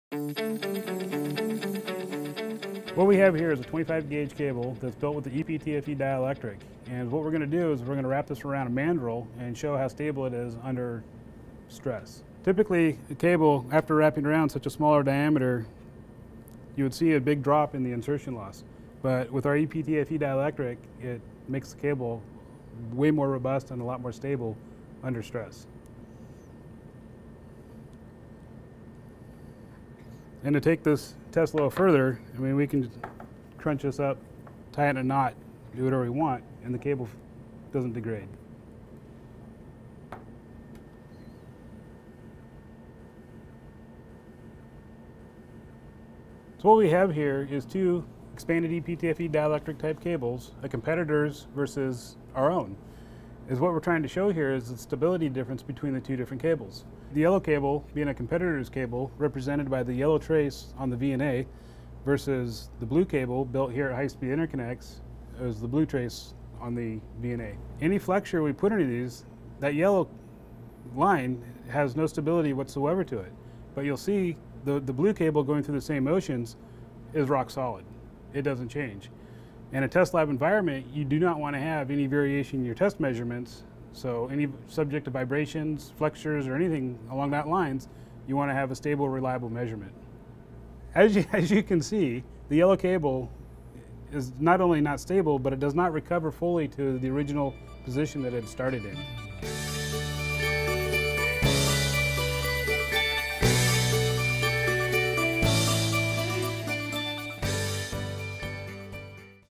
High Speed Interconnects. Highly Stable. Highly Flexible.
In this video we demonstrate how incredibly stable HSI cables with vP90 ePTFE truly are. First we wrap a cable around a mandrel and demonstrate zero degradation in insertion loss. Next, we compare our cable assembly against a competitors and demonstrate how the competitor fluctuates under duress while ours is rock steady.